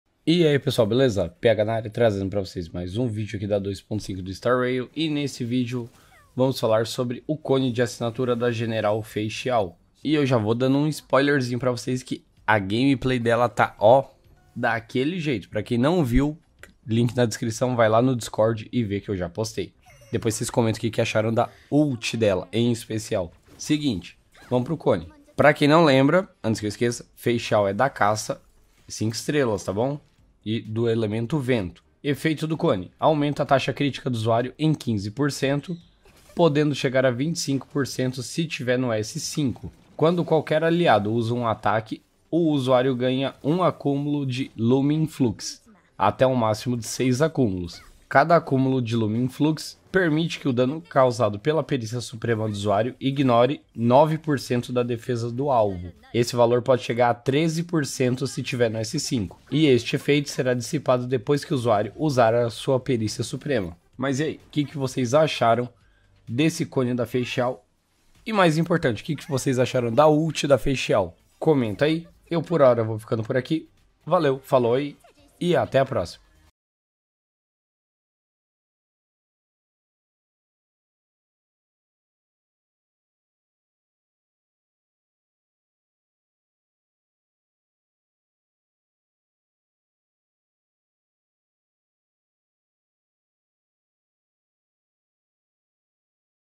Vazou! O Lightcone de Assinatura da Feixiao | Honkai Star Rail
Nesse vídeo bora falar da General Feixiao e como vai funcionar seu kit.

😍Código de Criador Epic: ralphs

🎥MEUS EQUIPAMENTOS
-2x Memória Kingston DDR4 4GB